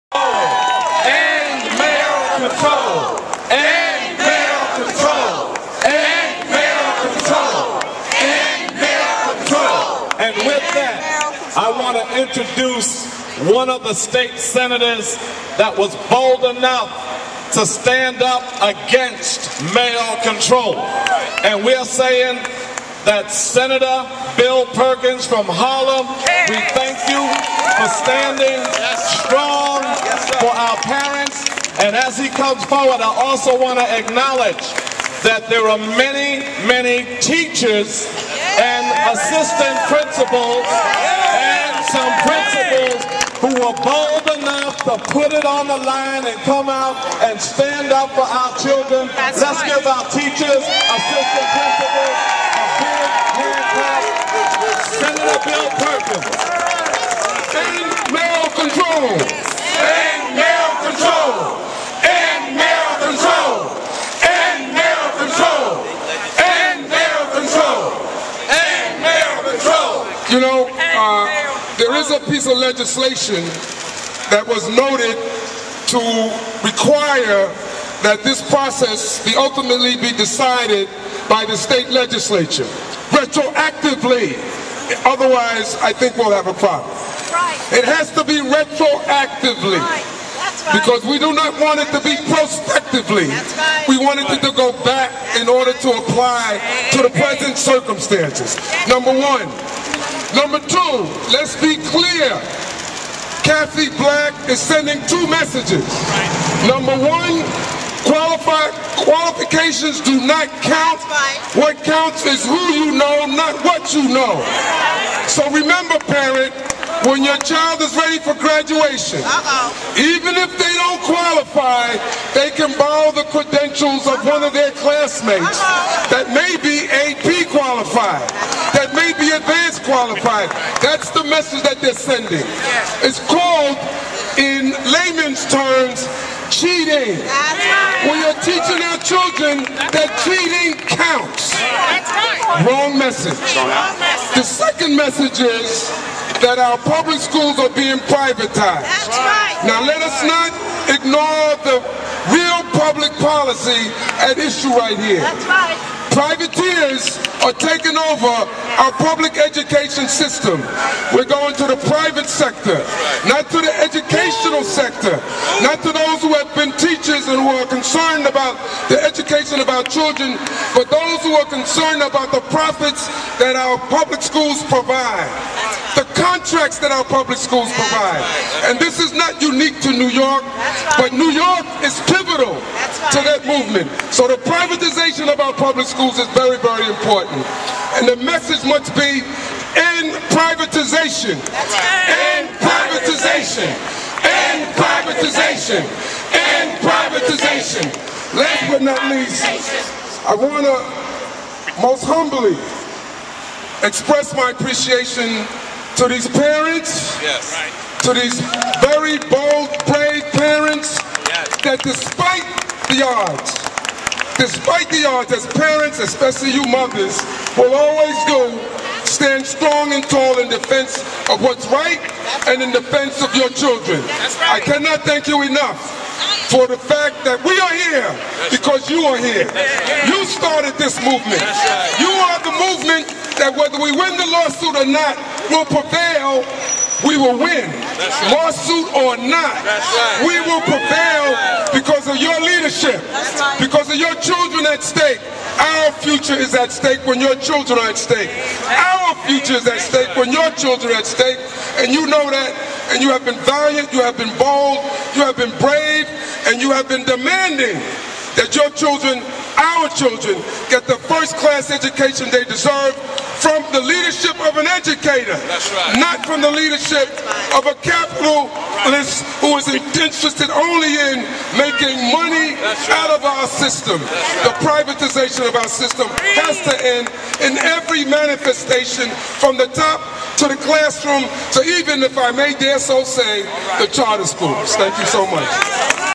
Senator Bill Perkins@Deny Waiver.com Coalition
Recorded on December 2, 2010 Hip Pioneer Radio Rahim Reporting at tweed protest city hall nyc, parents, teachers fight back Here Senator Bill Perkins speaks for public education